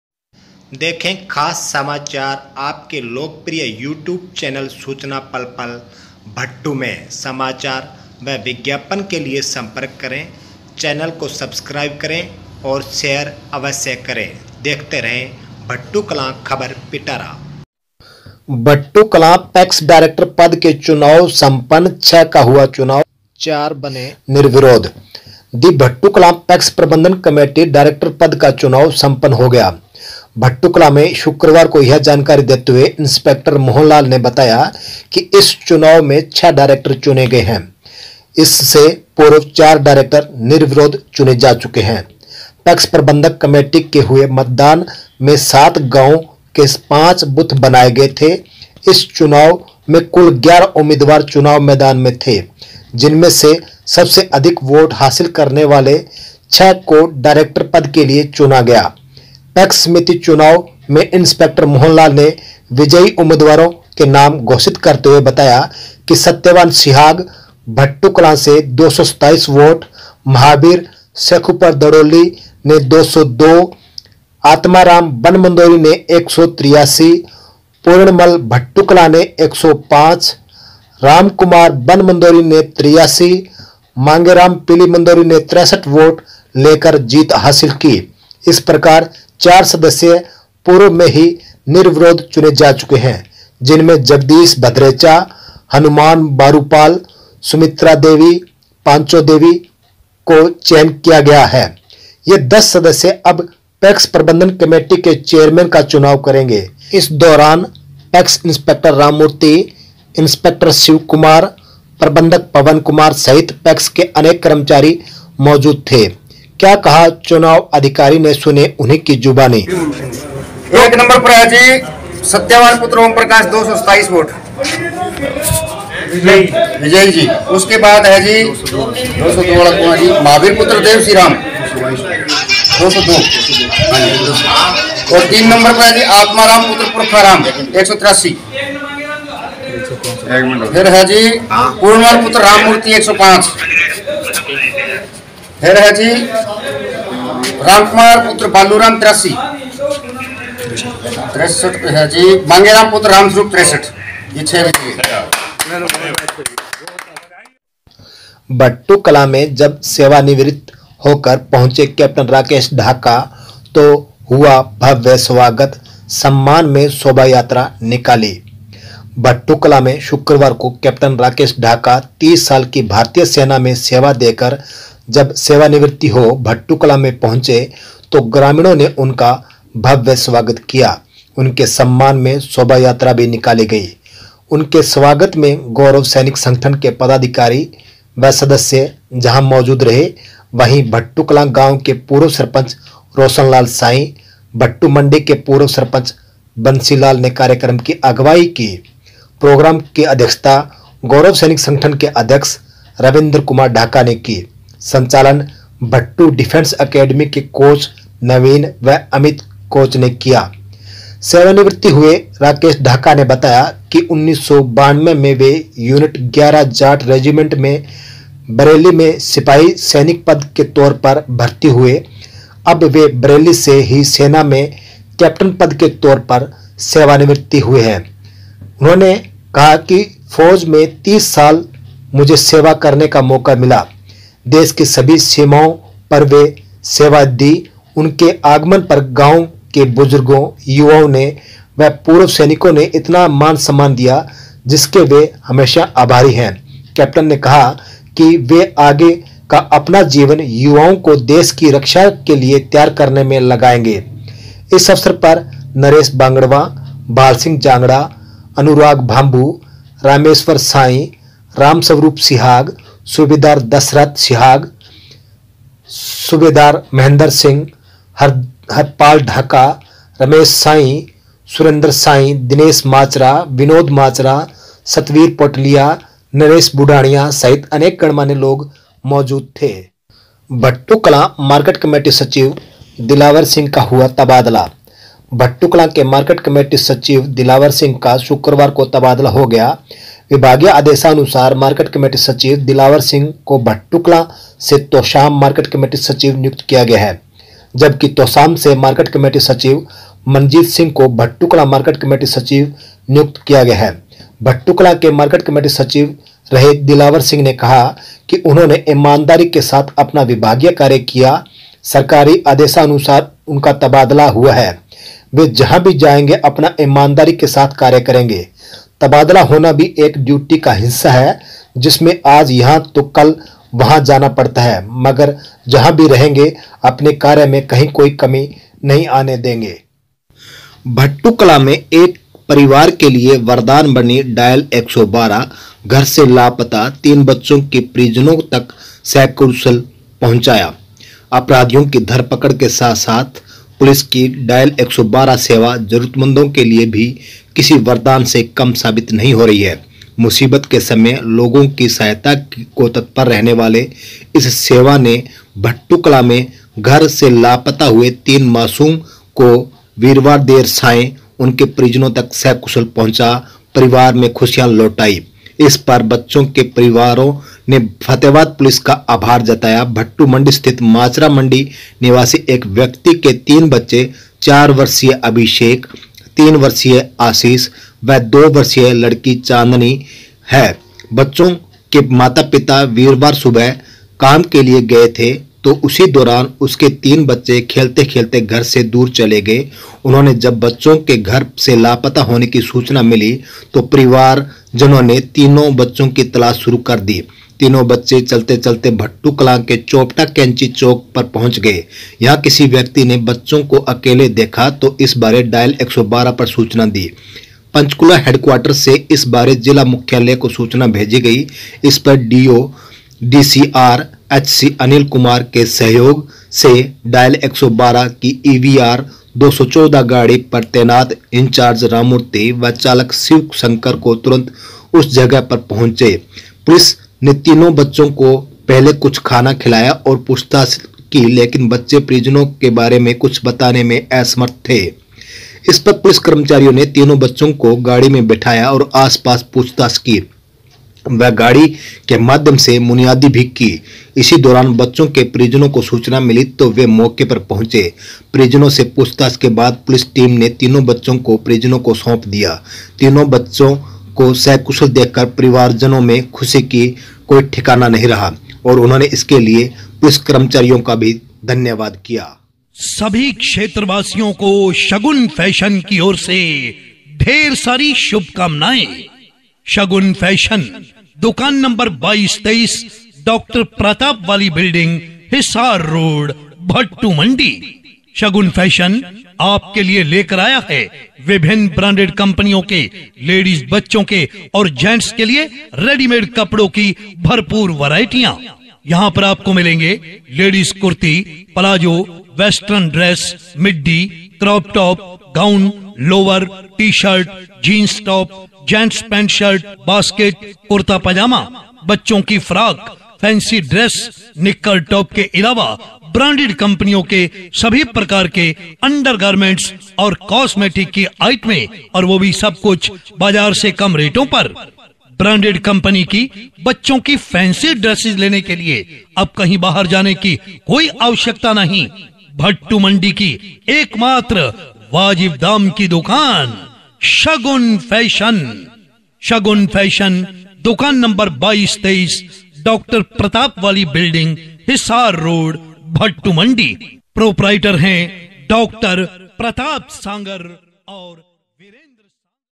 भट्टू न्यूज 4/03/2022 सत्यवान, महावीर सहित10 बने पैक्स निदेशक, कैप्टन राकेश ढाका का हुआ भव्य स्वागत,
भट्टू न्यूज 4/03/2022 सत्यवान सिहाग, महावीर बरड़वा सहित 10 बने पैक्स निदेशक, कैप्टन राकेश ढाका का हुआ भव्य स्वागत, लापता 3 बच्चों को सकुशल परिवार तक पहुंचाया। आपके पास भी कुछ है खास तो हमारे पास भेजे।